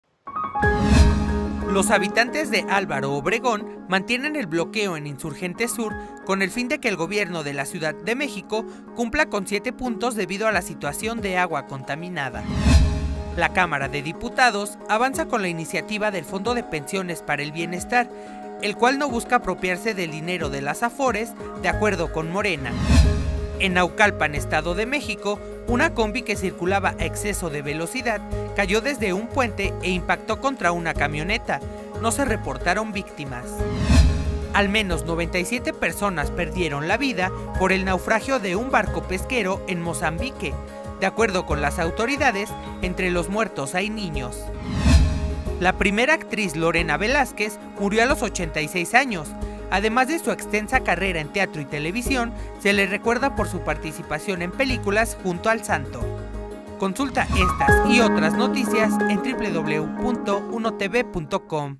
Las noticias de hoy, 12 de abril de 2024, en un minuto, hasta las 14:00 horas
0:00 ✅Bloqueo por agua en CDMX
0:12 ✅¿No se apropia de Afores?
0:22 ✅Combi cae sobre camioneta
0:33 ✅97 muertos por naufragio
0:44 ✅Muere la actriz Lorena Velázquez